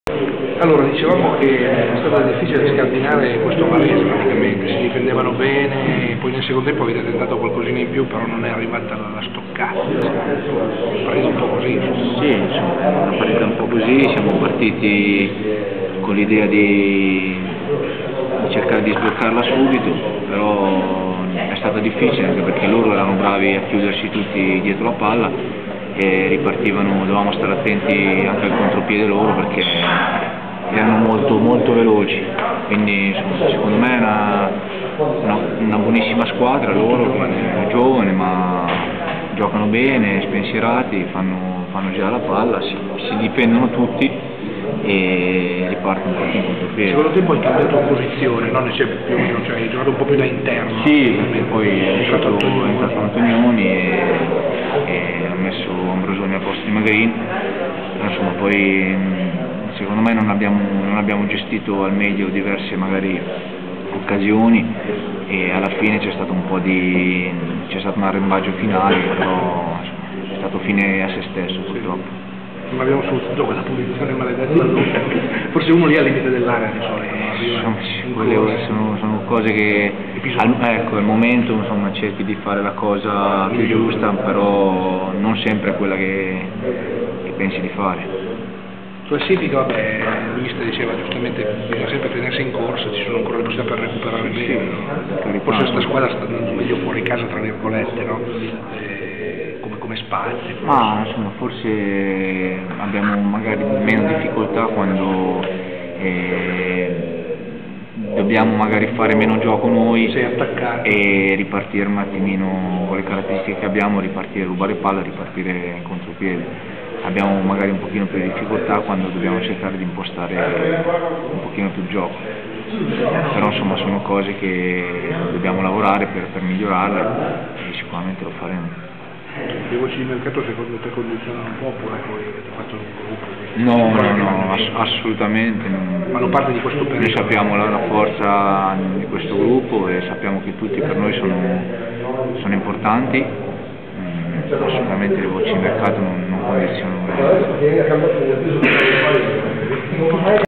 02 Pro Vercelli-Varese 0-0 campionato 2007/08
Il centrocampista della Pro Vercelli Davide Tedoldi analizza la partita casalinga contro il Varese terminata 0-0